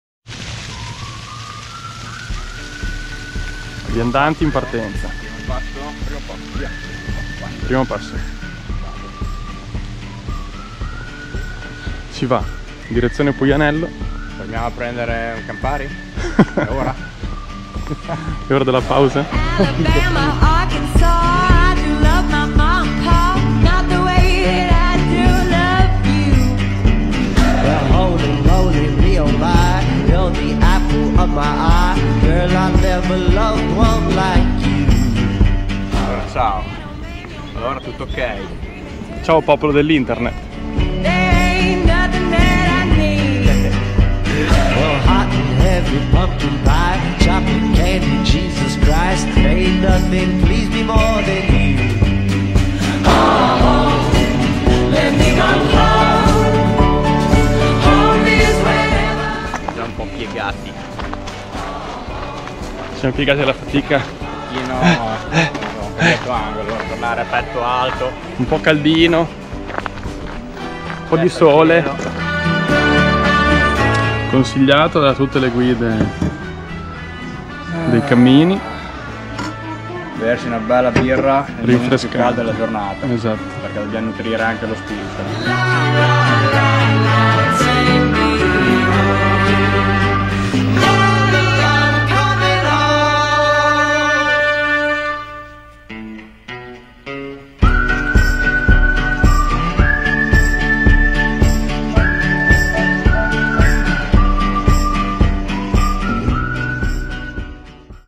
Via Vandelli Tappa 1: Modena - Puianello
In una settimana percorreremo i 176 Km che da Modena ci porteranno fino a Marina di Massa a toccare il Mar Tirreno, seguendo la via settecentesca ideata dall'abate Domenico Vandelli per collegare la capitale del ducato di Modena con il mare.